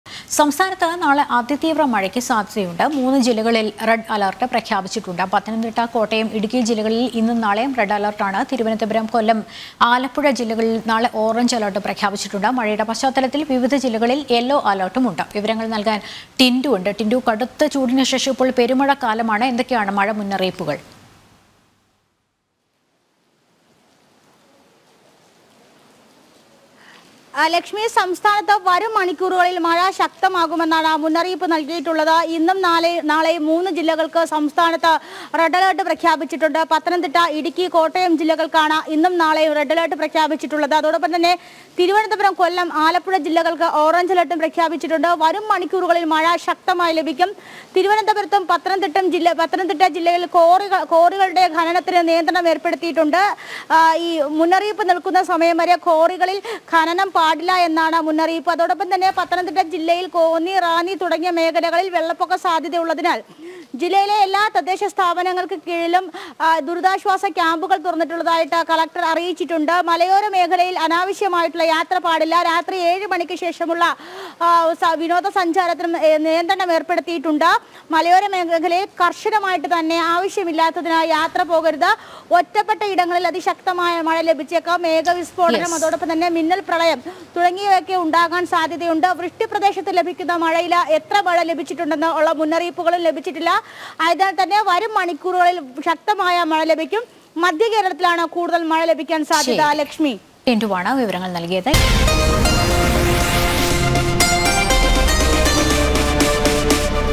സംസ്ഥാനത്ത് നാളെ അതിതീവ്ര മഴയ്ക്ക് സാധ്യത | RAIN
സംസ്ഥാനത്ത് നാളെ അതിതീവ്ര മഴയ്ക്ക് സാധ്യത. മൂന്ന് ജില്ലകളിൽ റെഡ് അലർട്ട് പ്രഖ്യാപിച്ചു. പത്തനംതിട്ട, കോട്ടയം, ഇടുക്കി ജില്ലകളിൽ ഇന്നും നാളെയും റെഡ് അലർട്ട്. തിരുവനന്തപുരം, കൊല്ലം, ആലപ്പുഴ ജില്ലകളിൽ നാളെ ഓറഞ്ച് അലർട്ടും പ്രഖ്യാപിച്ചിട്ടുണ്ട്. മഴയുടെ പശ്ചാത്തലത്തിൽ വിവിധ ജില്ലകളിൽ യെല്ലോ അലർട്ടും പ്രഖ്യാപിച്ചിട്ടുണ്ട്.